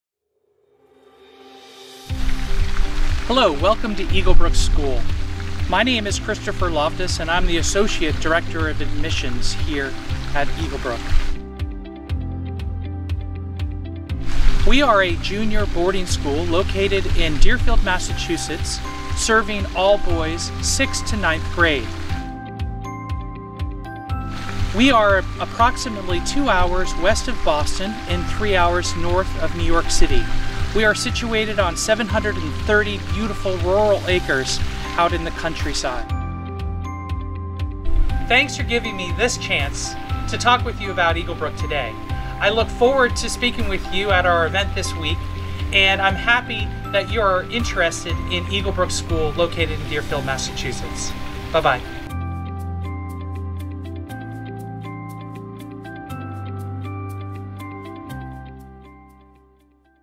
Приглашение от Eaglebrook School
"Регистрация Представитель Eaglebrook School Кристофер Лофтус приглашает вас на День Американских Школ в StudyLab. Приходите всей семьей! Когда: 28 октября (суббота), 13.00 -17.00 Где: Никольская 10, БЦ «Никольская Плаза», Образовательное пространство, 4 этаж. Обучение за рубежом со Studylab